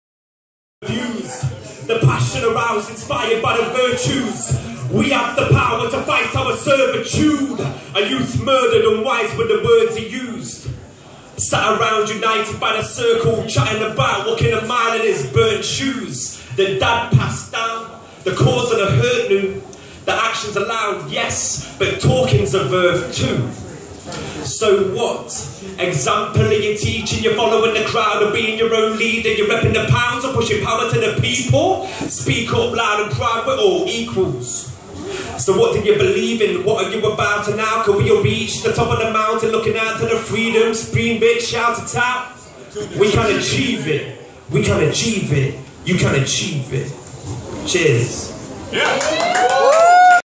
...pr@xis... performing at Hairy Dog 9/8/14
As part of the Derby Dreams zine launch and Astronautalis gig host ...pr@xis... performs spoken word